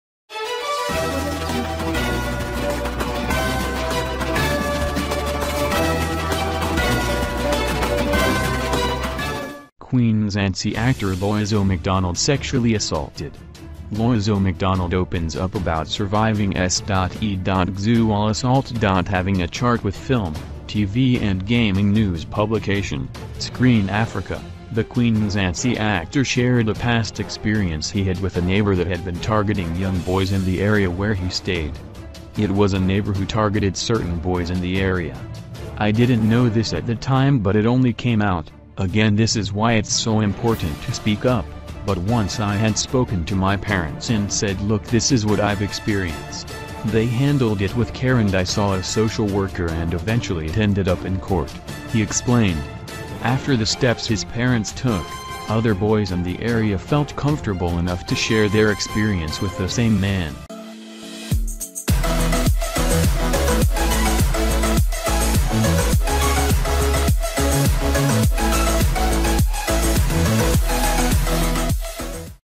Queen Mzansi actor Loyiso MacDonald Sexually Assaulted
Queen Mzansi actor Loyiso MacDonald Sexually Assaulted - Loyiso MacDonald opens up about surviving s.e.xual assault.Having a chart with Film, TV & Gaming news publication, Screen Africa, the Queen Mzansi actor shared a past experience he had with a neighbor that had been targeting young boys in the area where he stayed.